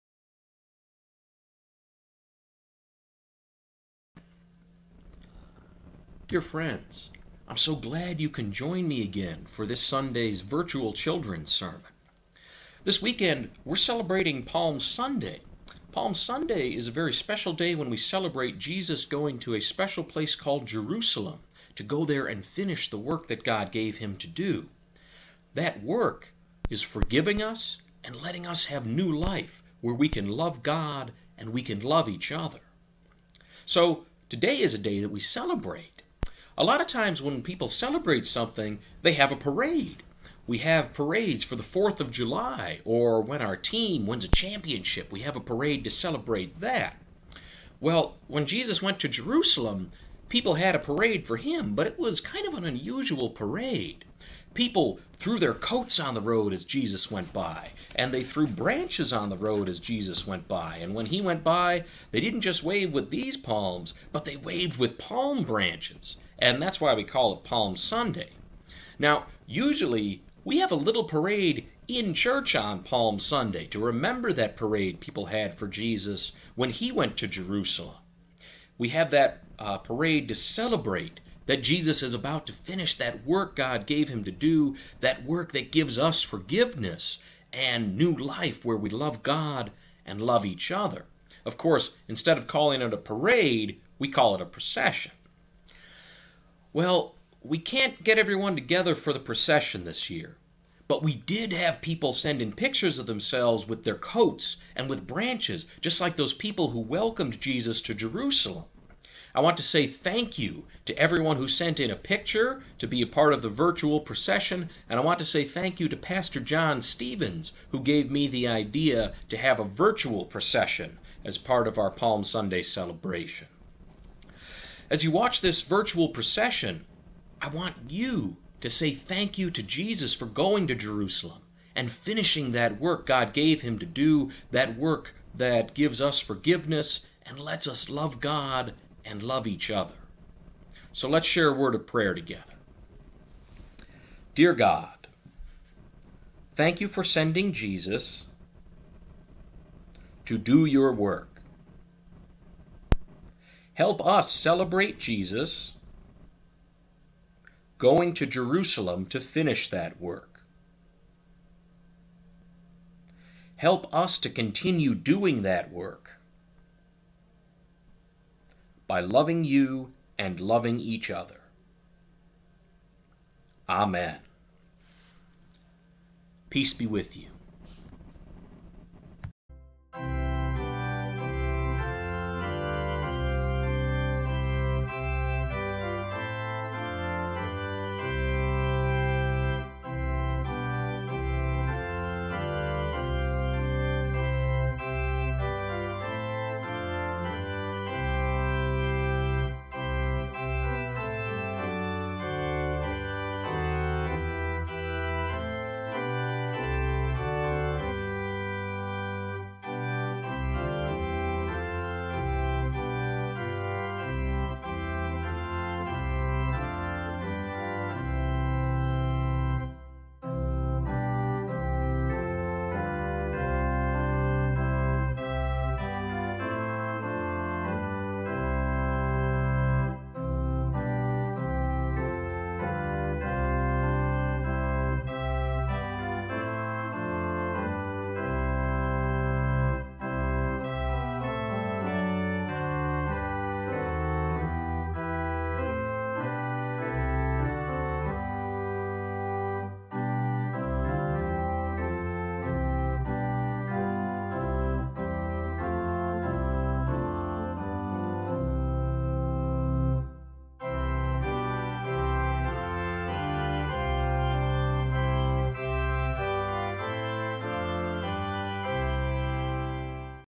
Children's Sermon, Palm Sunday, 4-5-2020
This children's sermon talks about Jesus going into Jerusalem and the celebration people had for him when he did.  Thank you to everyone who sent in a picture for this.  Enjoy the virtual procession!